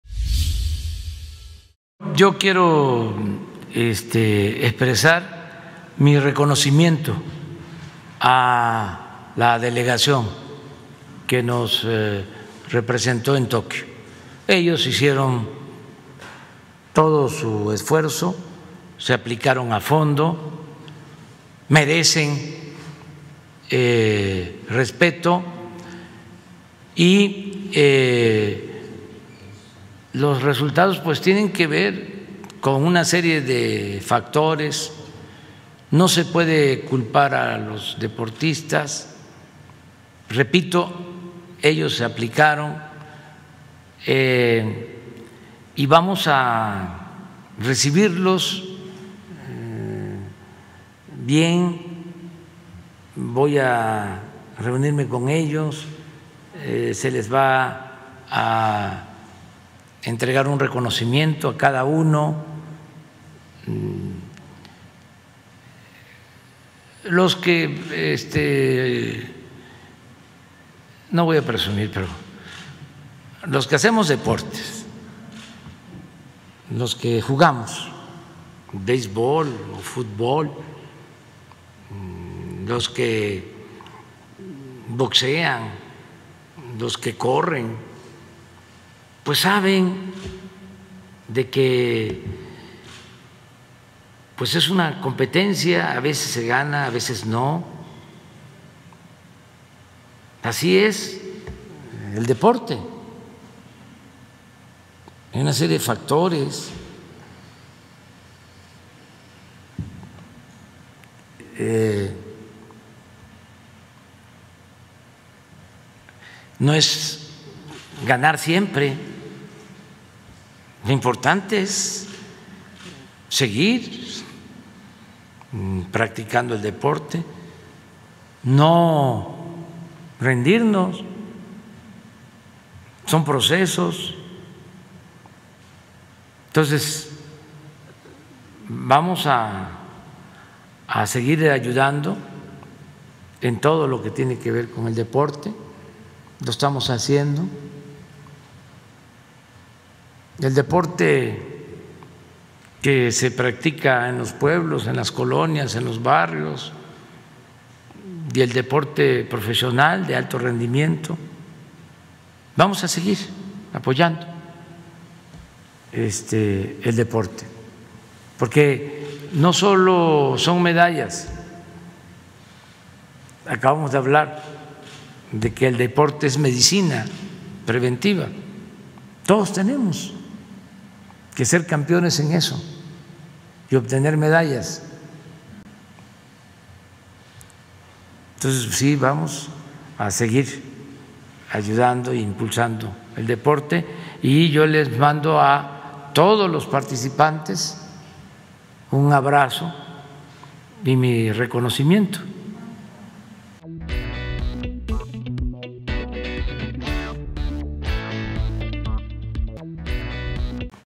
AMLO exculpa a deportistas mexicanos por los resultados en Tokio 2020
El presidente de la República Mexicana exculpó a la Delegación Mexicana que participó en los Juegos Olímpicos de Tokio por los cuatro bronces conseguidos